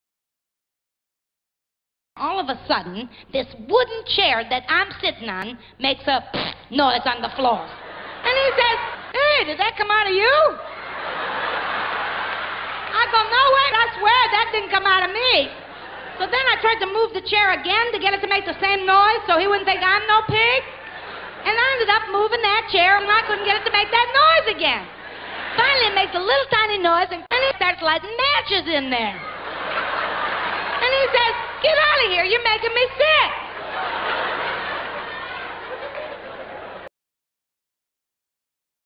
The Mona Lisa Speaks and Shares An Embarrassing Secret
now we know why she neither smiles nor frowns